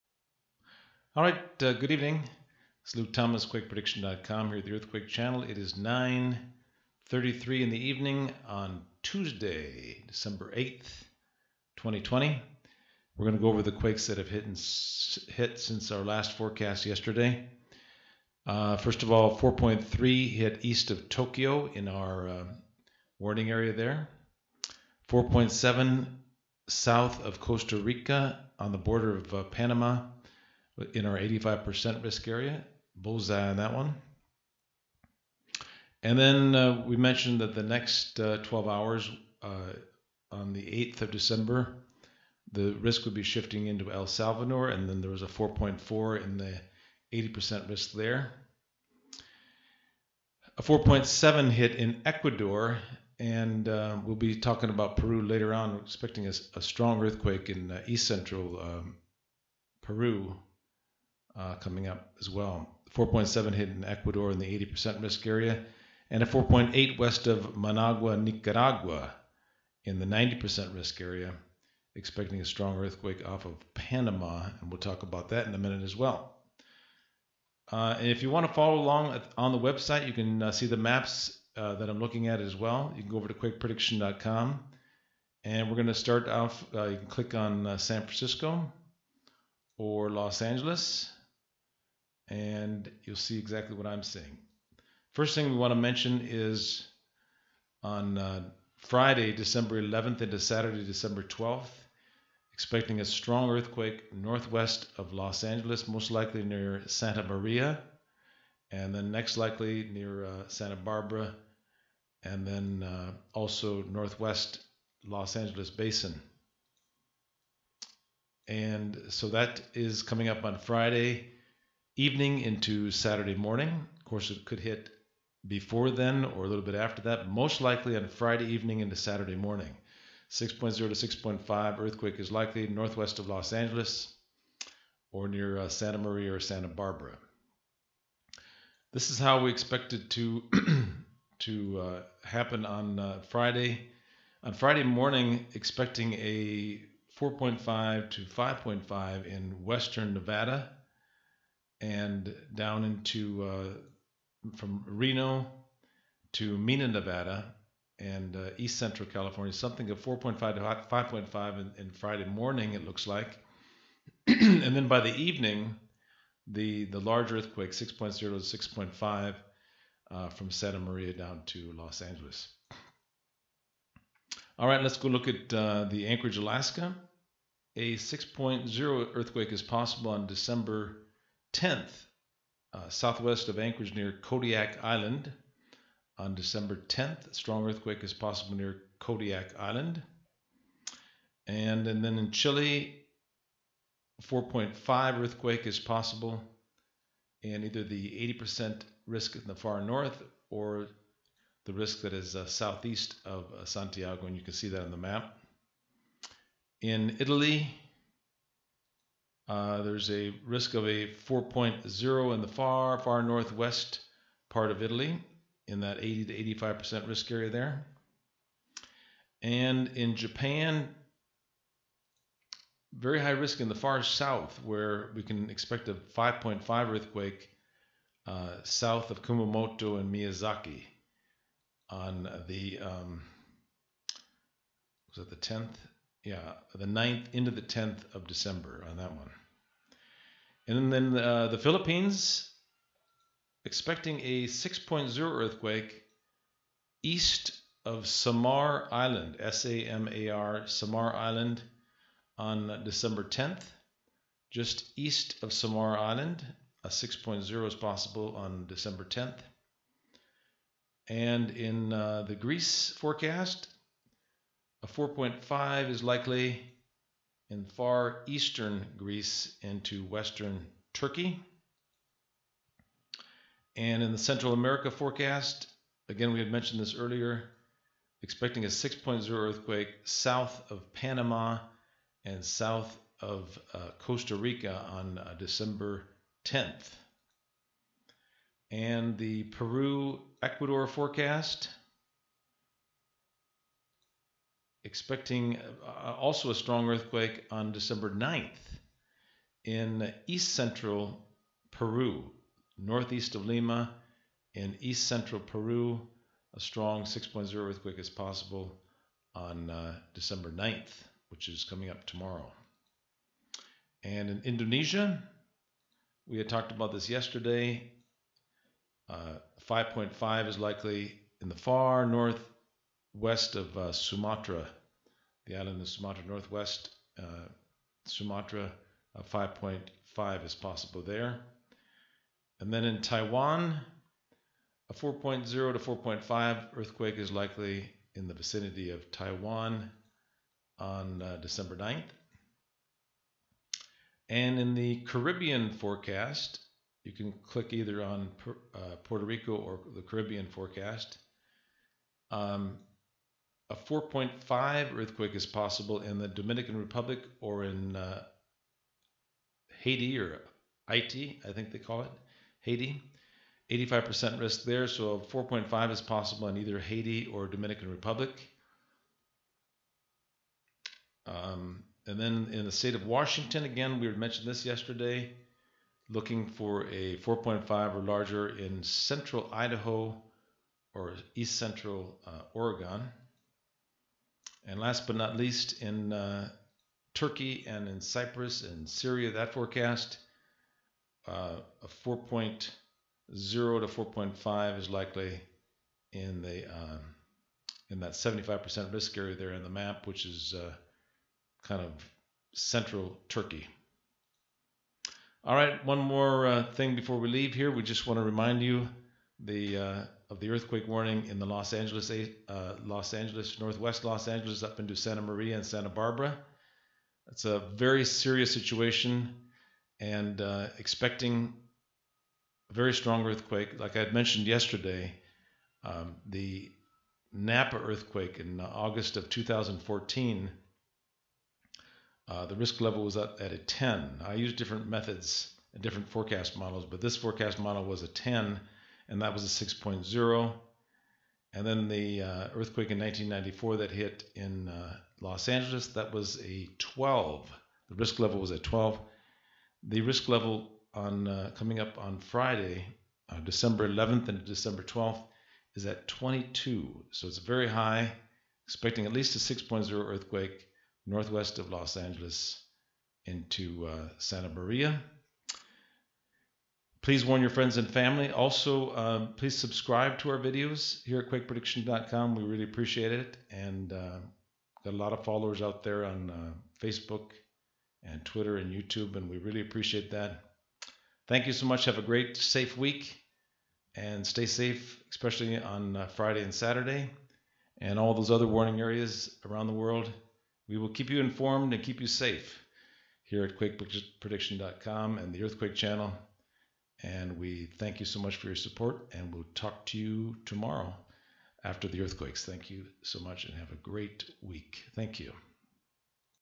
Los Angeles Earthquake Warning 6 5 earthquake 12 11 12 12
WARNING: 6.5 earthquake is likely NW of Los Angeles on 12/11 or 12/12 - Please warn friends and family